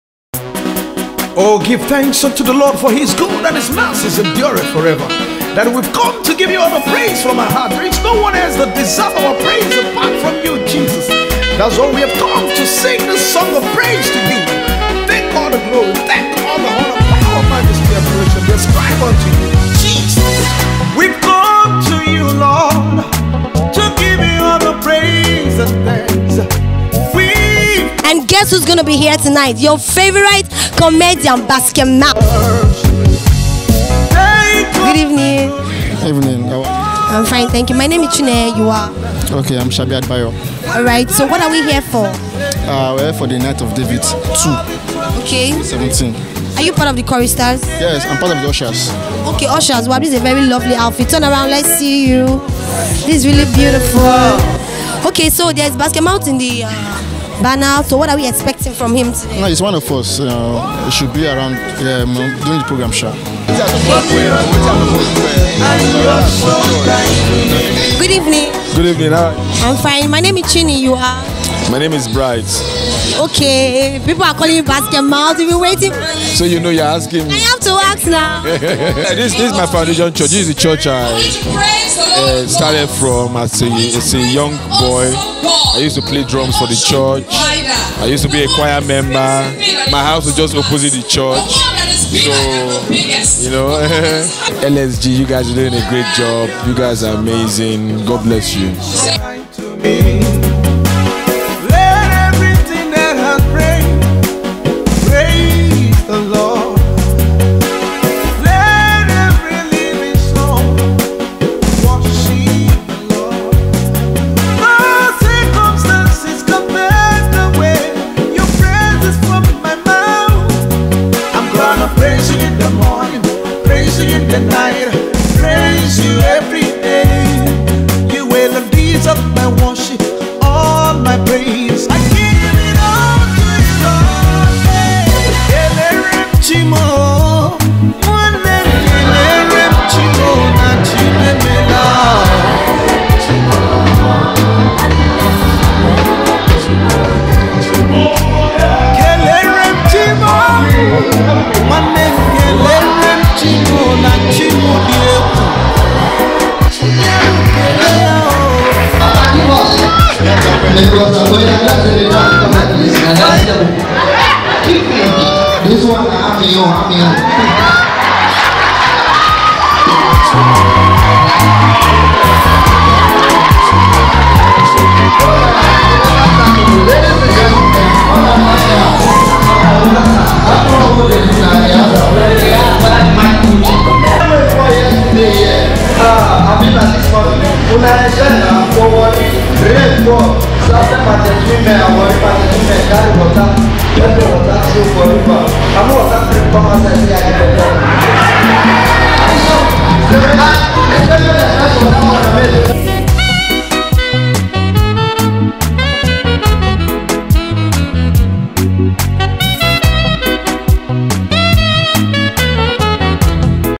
BASKET MOUTH in Kirikiri Town Lagos Nigeria covered by LXG MEDIA
Bright popularly known as BasketMouth CAME TO HIS HOME CHURCH HERE IN LAGOS, TO WORSHIP GOD IN A CHURCH WHERE HE WAS ONCE A DRUMMER BOY IN THE CHOIR some years back.